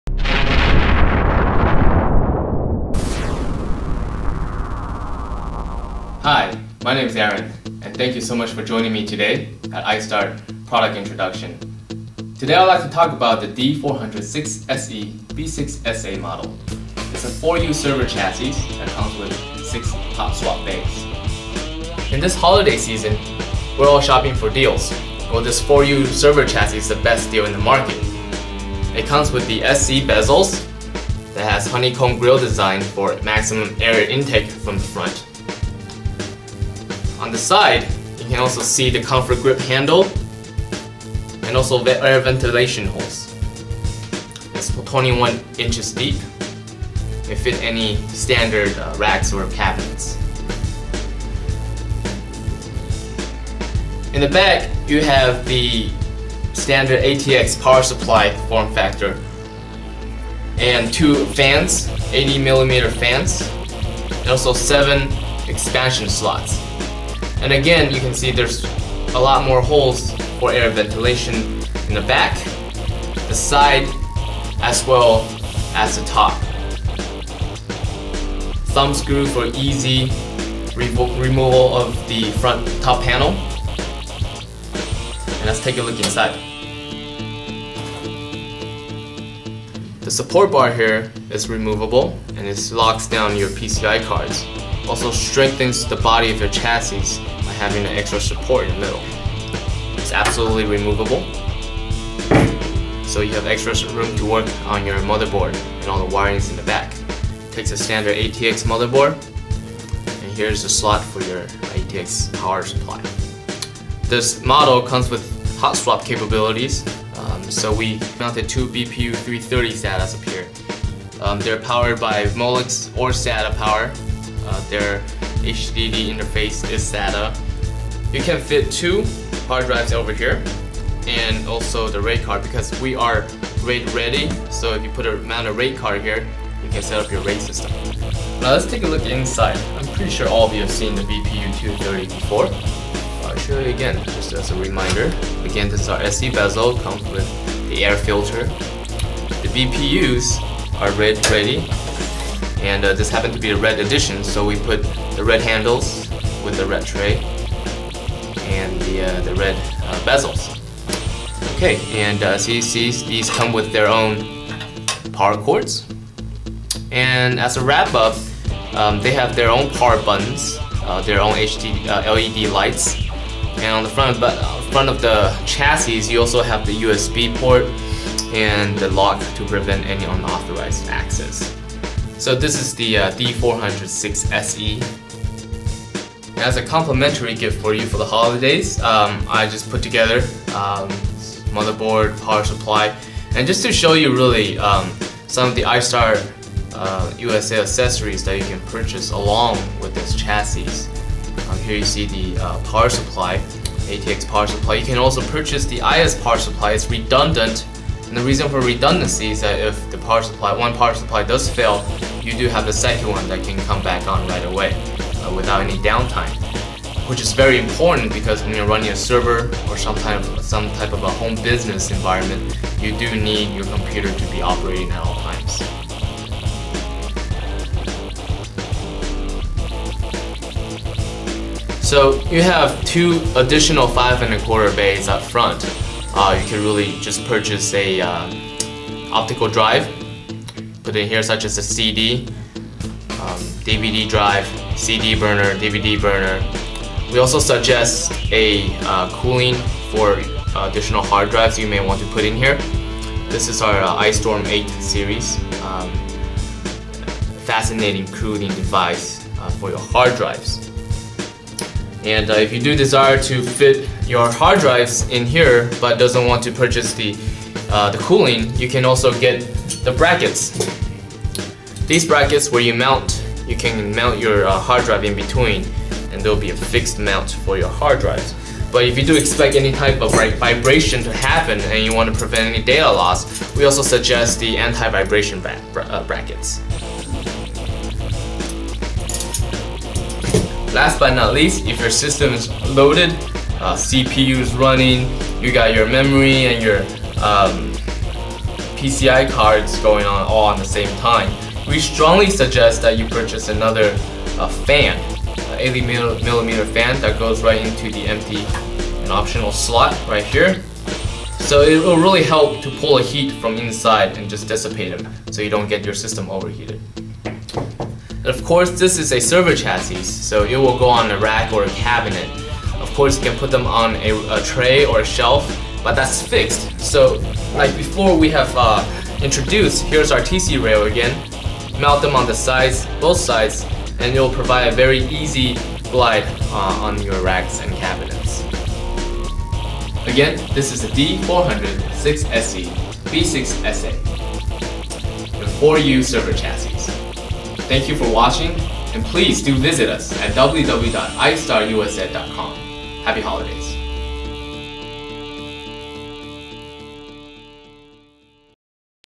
How to build your server around the D-400-6SE-B6SA Rackmount Chassis
iStarUSA 4U industrial rackmount chassis D Storm D-400-6SE-B6SA can be combined with BPU Series to enable hot-swappable storage capability. It also supports up to seven add-on cards and that has the potential for a total storage capacity up to 6 hot swap storage drives for RAID configuration. D-400-6SE-B6SA provide your server or cluster maximum reliability and performance. Optional Aluminum Body reduces total chassis weight and also provides enhanced thermal cooling solution.